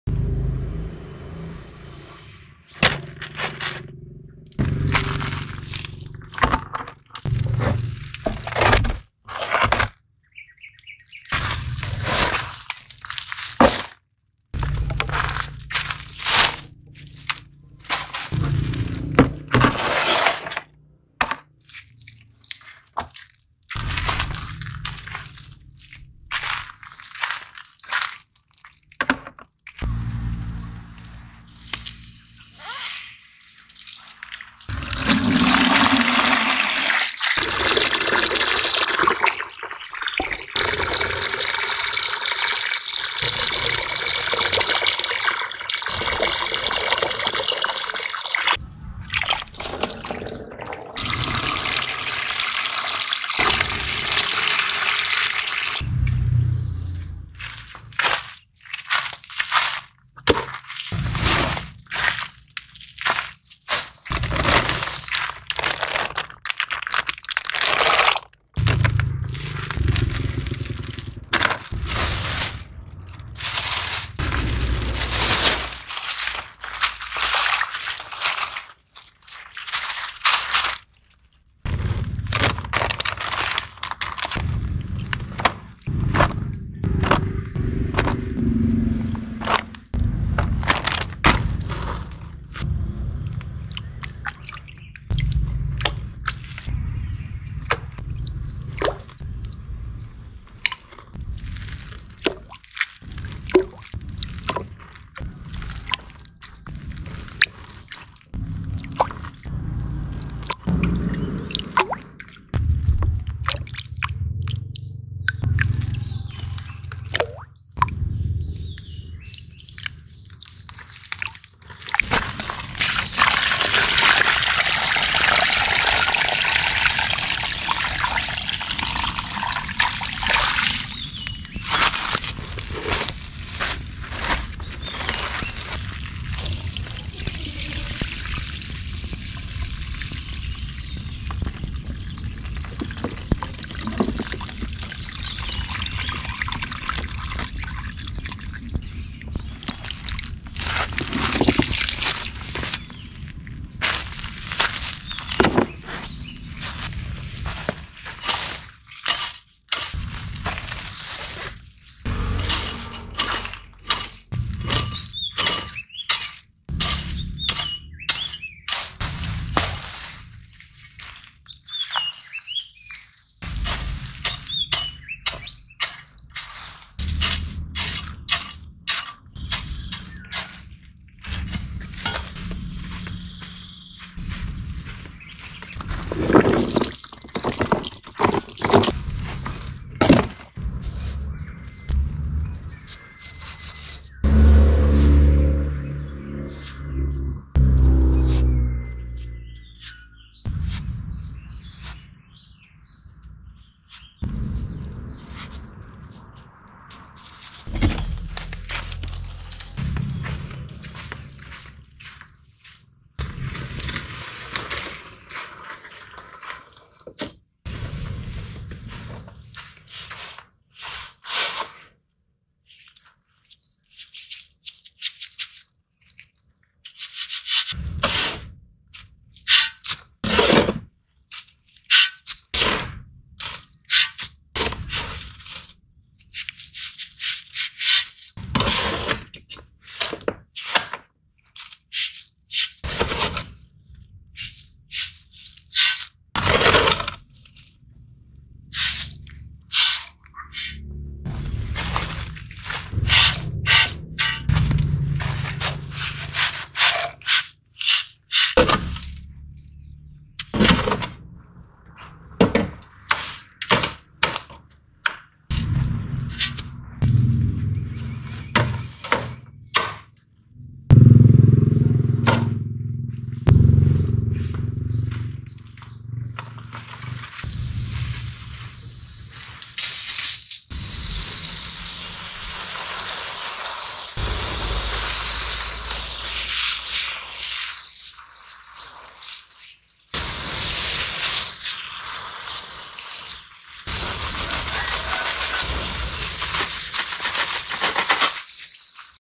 ASMR semai biji durian
kita buat ASMR semai biji durian, selalu tengok orang makan kan.